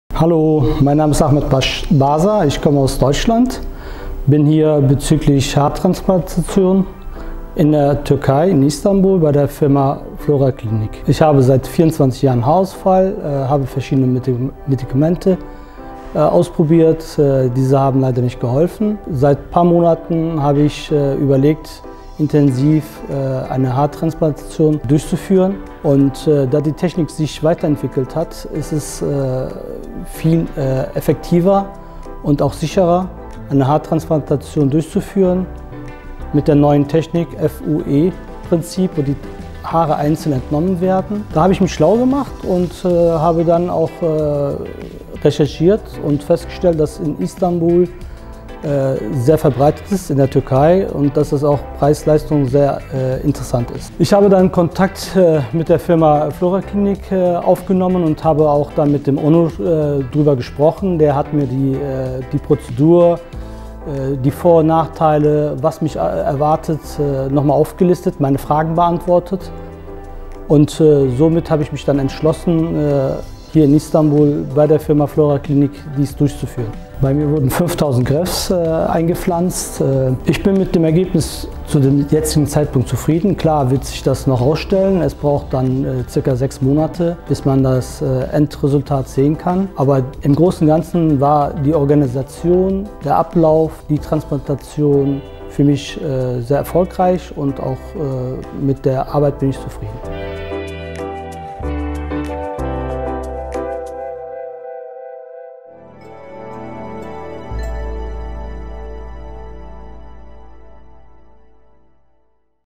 Saç Ekimi Danışan Yorumu | Flora Klinik İstanbul 2021
Almanya'dan İstanbul'a gelerek kliniğimizde FUE Saç Ekimi yaptıran danışanımız Ahmet Bey'in sürece ilişkin yorumları.